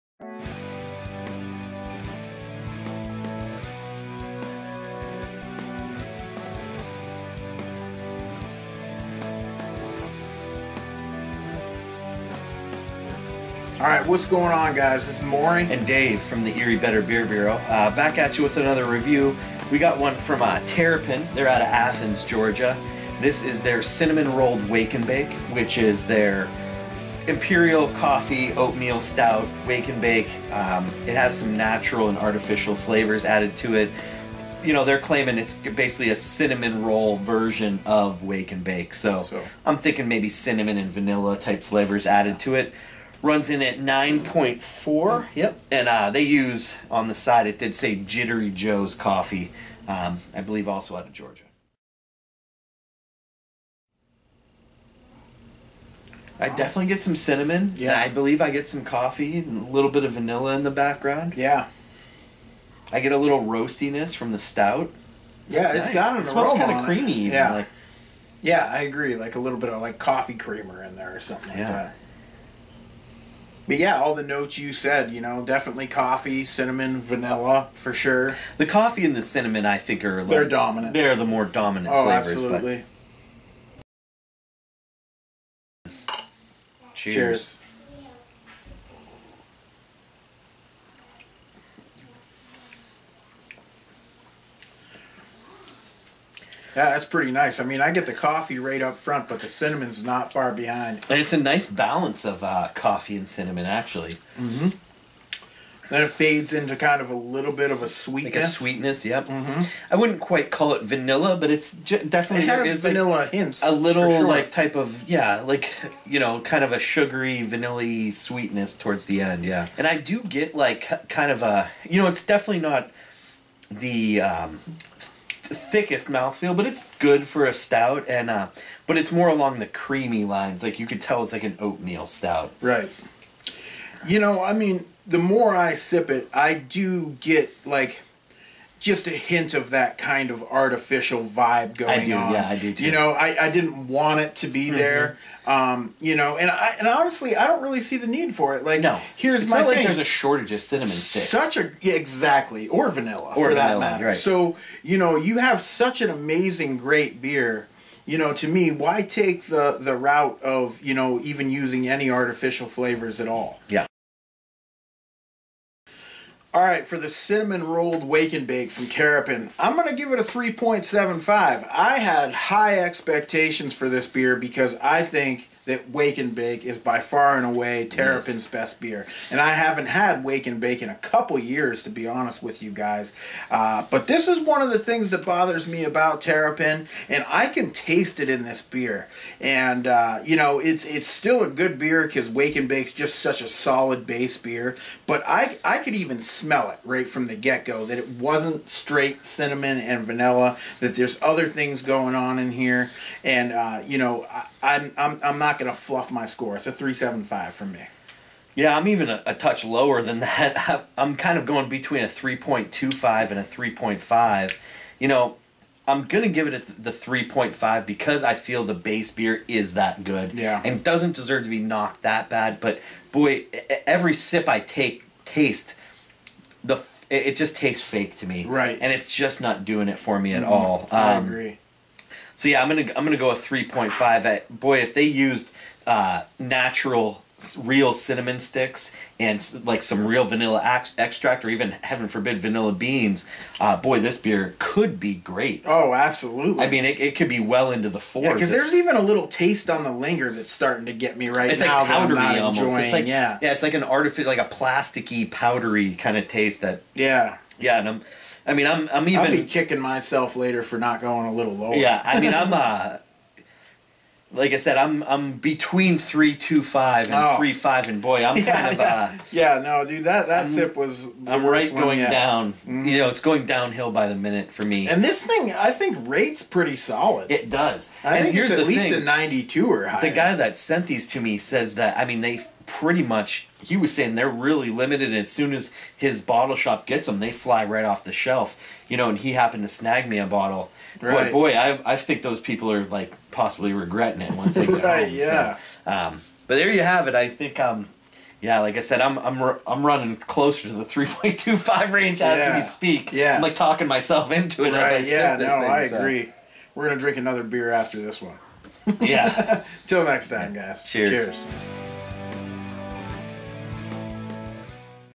EBBB: Terrapin Cinnamon Roll'd WnB - Review #366
Description: Like your favorite breakfast treat and a great cup of coffee, Cinnamon Rolled Wake-n-Bake hits on all cylinders. Starting with a delicious coffee aroma, a touch of Cinnamon gives way to a sugary sweet icing flavor as a buttery nuttiness fills out the last drop of this breakfast in a glass.